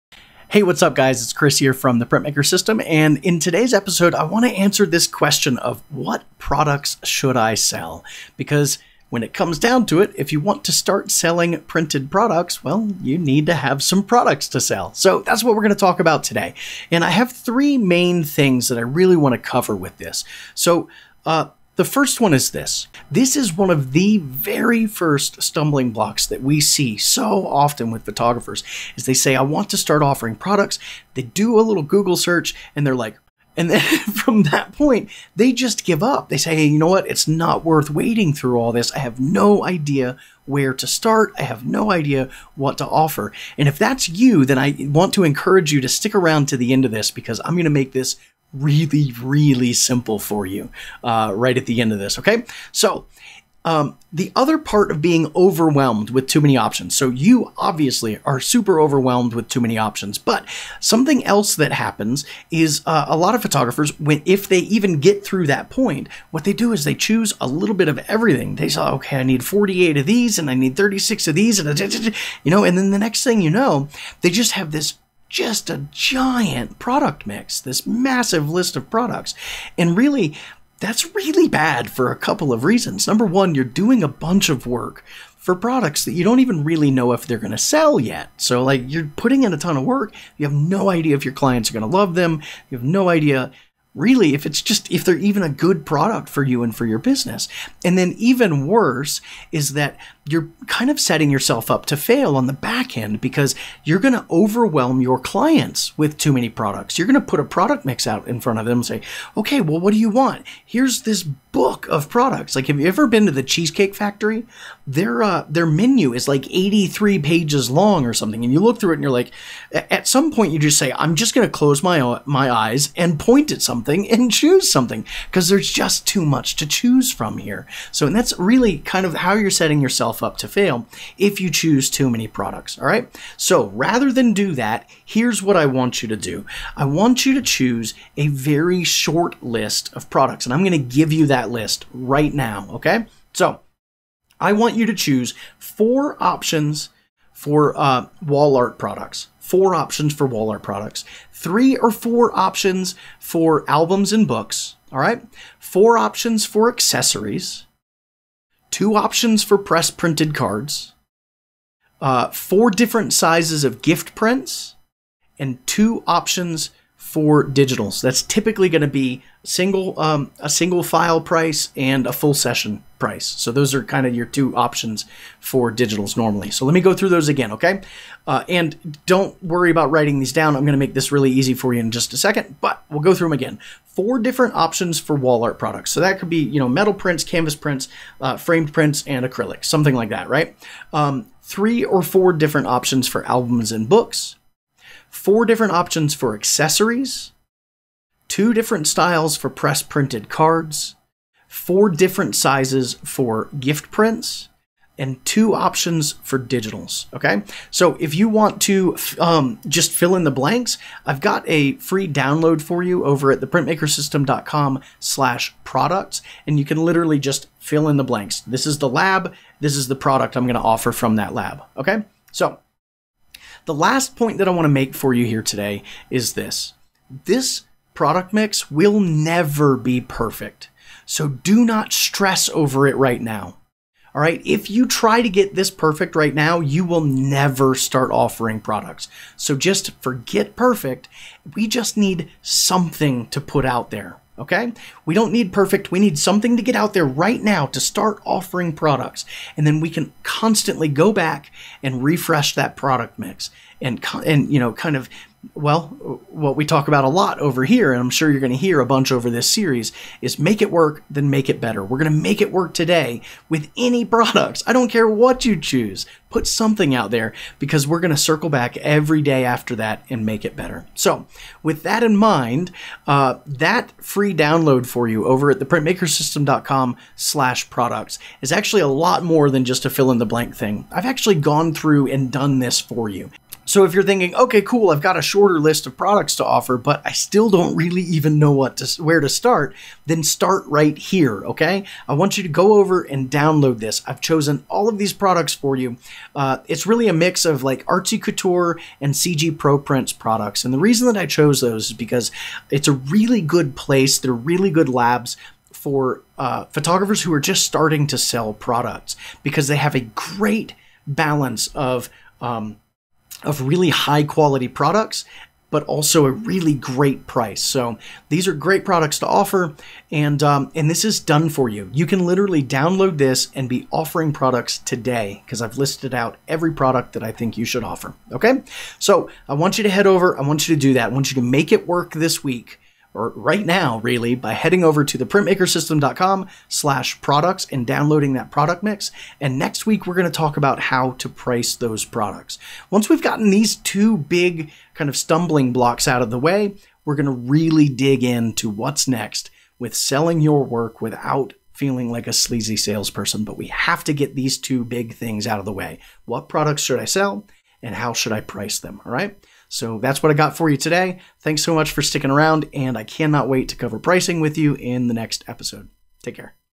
IPS Redefined, Pt 02: What Photography Products Should I Sell?
Professional photographers - overwhelmed by all of the print product options out there? Just want someone to tell you what you should sell, so you can move on and start selling?

This episode of "IPS, Redefined" is for you!

In this quick episode, you'll learn:

- Why too many photography product options isn't just bad for you, it's bad for your clients, too
- How many of which print product types you should offer
- The shortcut for hitting the ground running TODAY with a fully finished product mix